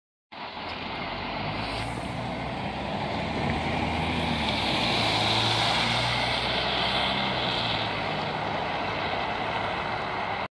Qbuzz Streekbus 3279 als Lijn 75 naar Station Emmen
Ritnunmer 24 2e Exloërmond Nijverheidsstraat - Exloo Hoofdstraat - Station Emmen